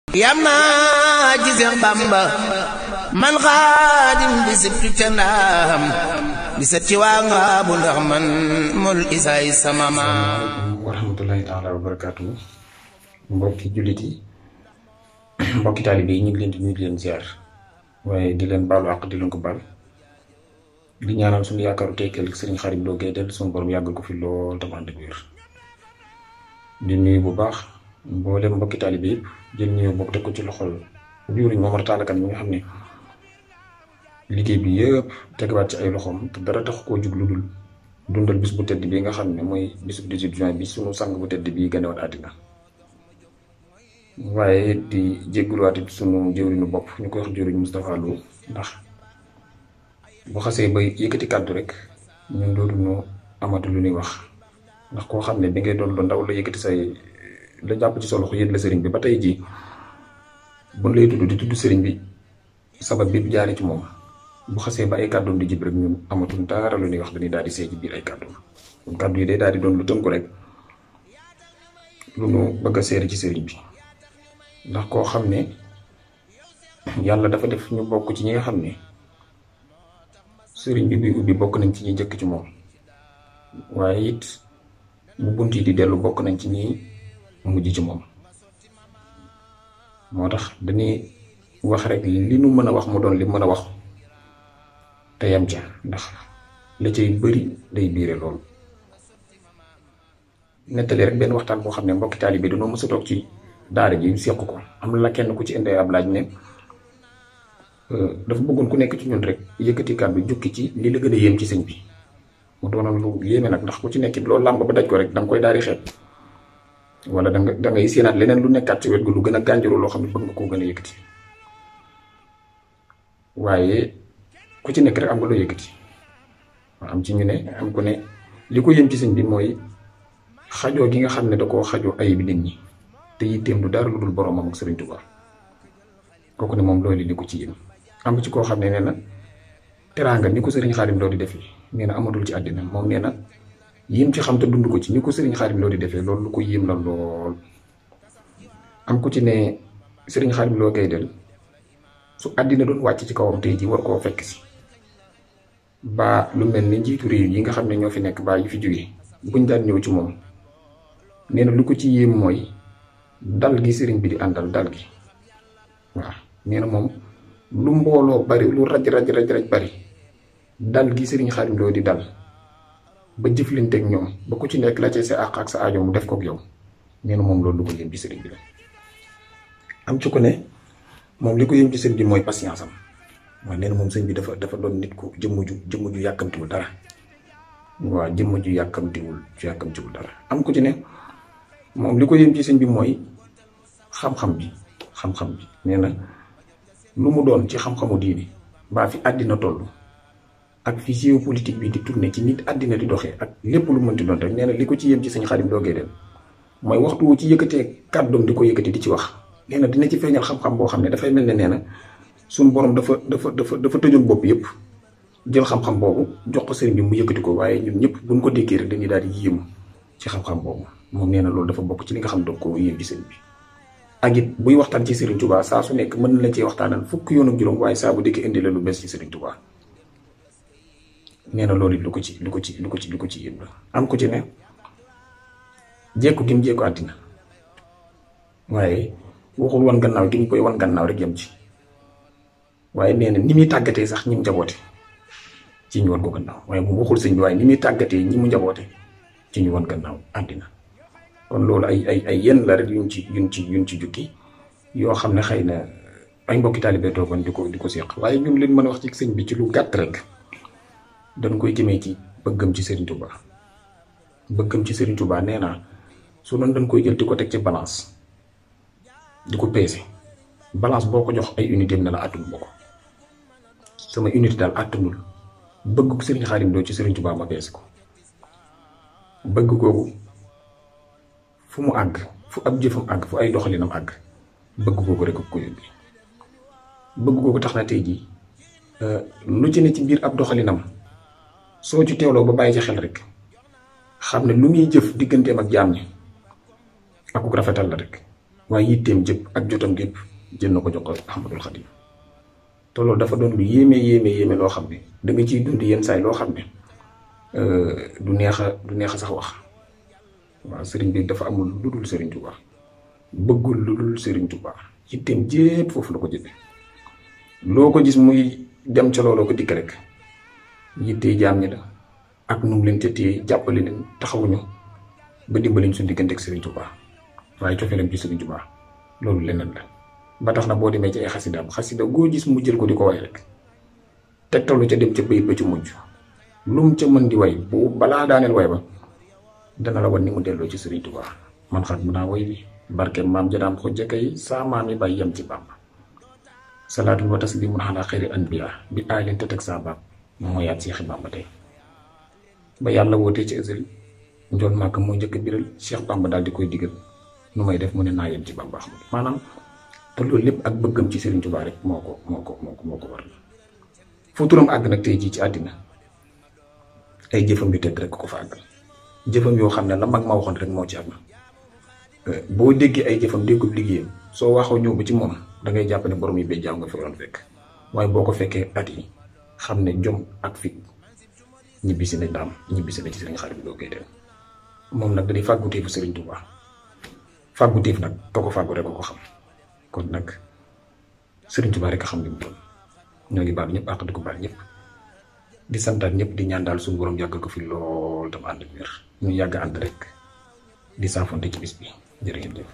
Special 18 Juin 2021 - Témoignage de Dieuwrigne NDiouga LO Bakhdad
Visitez le site web khadimrassoul.net - 1er Portail d'informations islamiques au Sénégal